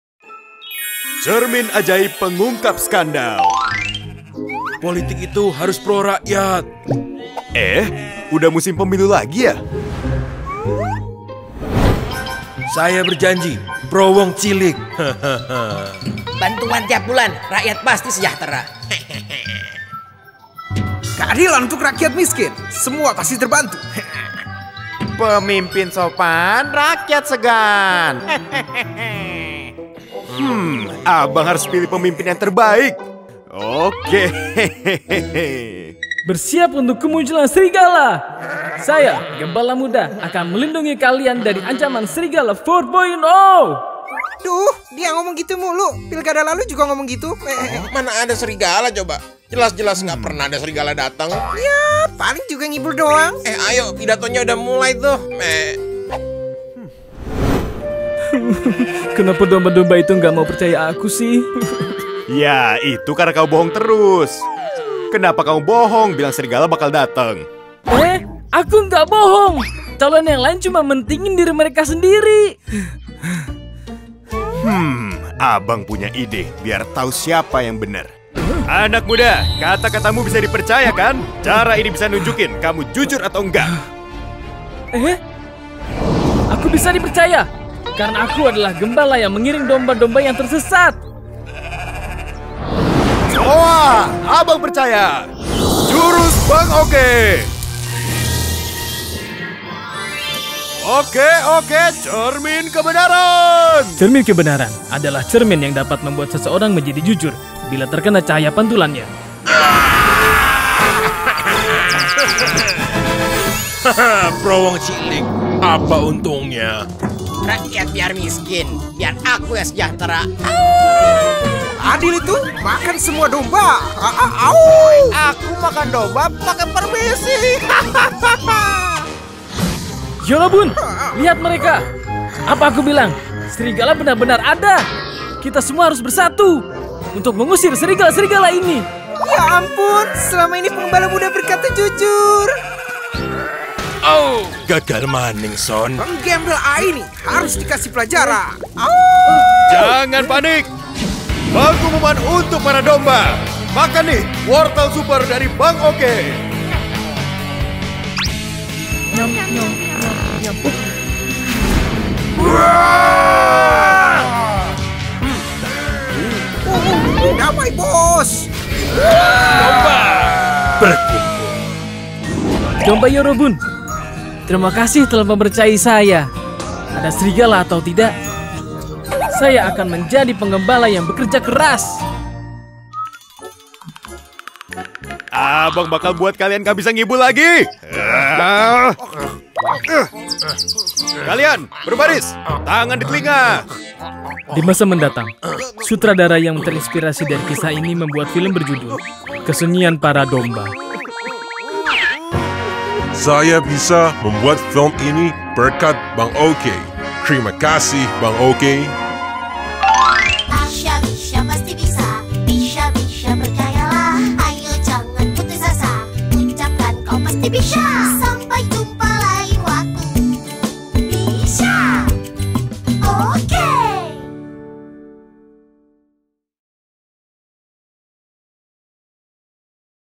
Siasat Licik Politik! Demi Menangkan Pemilu Kepala Desa Tipu Rakyat
Kalau musim pemilu, sering banget nggak sih dengar janji manis dari calon pemimpin? Kira-kira kalian percaya nggak nih sama janji mereka? Hmm kira-kira cuma janji manis di mulut aja atau memang benar direalisasikan ya? Eits tenang, jaman udah canggih gini Bang OK punya alat buat mengungkap kebenaran. Jangankan janji calon pemimpin, janji mantan kamu aja bisa kedeteksi bener apa nggak🤭.

Penasaran kan apa sih alatnya? Cus deh tonton sampai habis biar kamu nggak terbuai dengan janji manis mantan. Eh salah maksudnya janji manis calon pemimpin😁